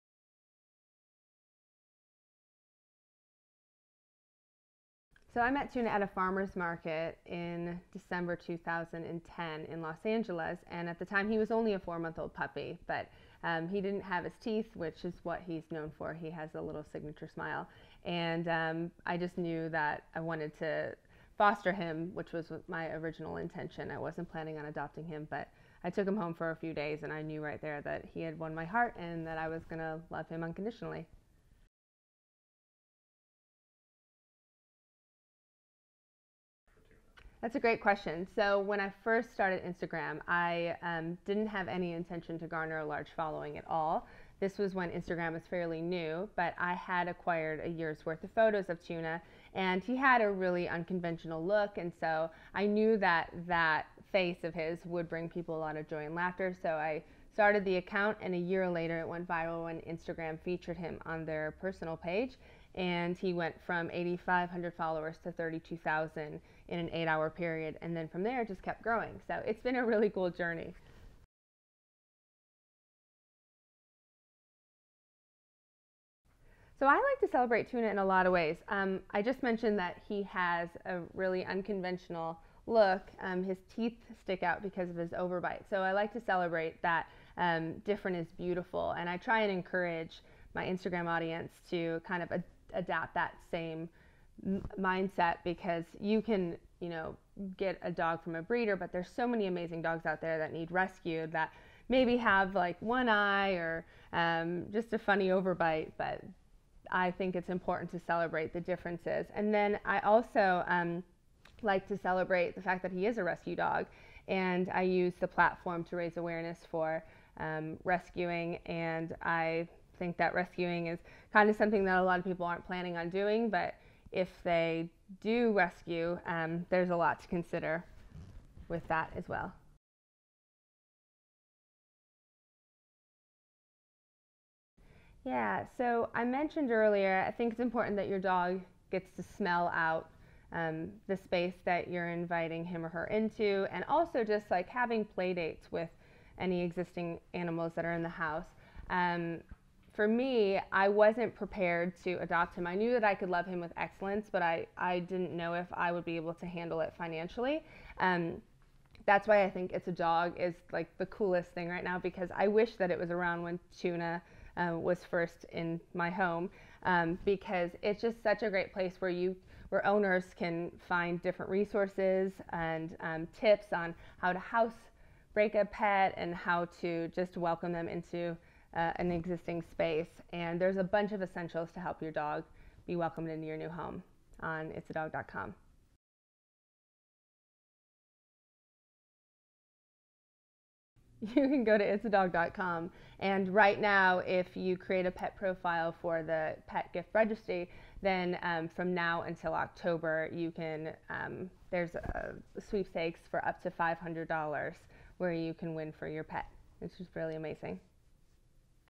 You Have a New Dog, Now What?
Adding a new dog to a household is an exciting event. Need some tips about bringing home your new friend? Courtney Dasher has some tips to share after all she’s mom to one of the hottest dogs on the internet – Tuna.